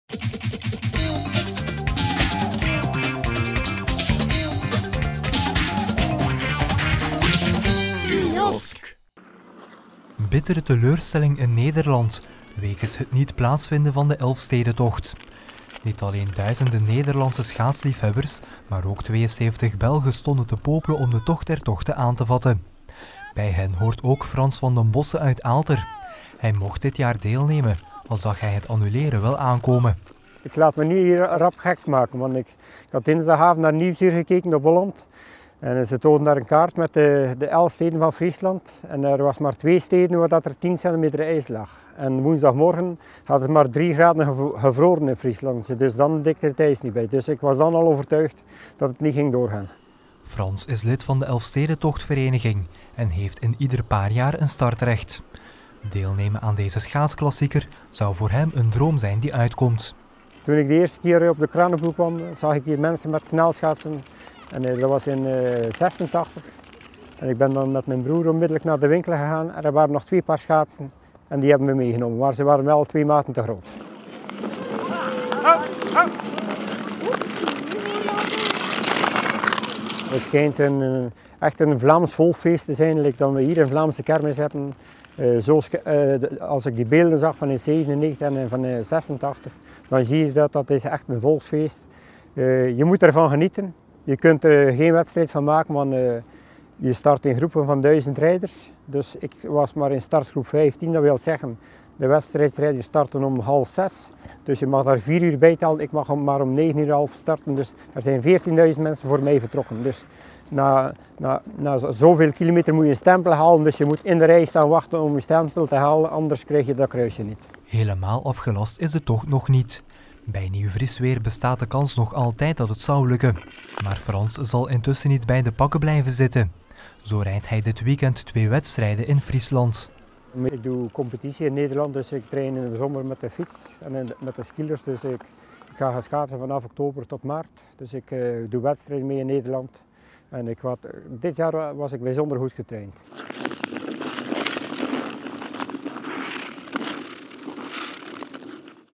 AVS Kiosk Elfstedentocht voor Frans Van den Bossche
Frans Van den Bossche uit Aalter was dit jaar uitgeloot om deel te nemen aan de Elfstedentocht.